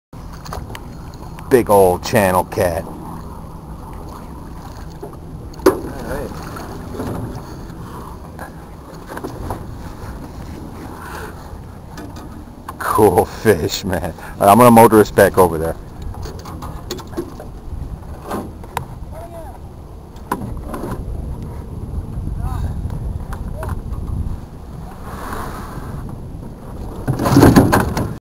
Channel Cat - Oops!
Weary angler unceremoniously plops his catch on the botom of boat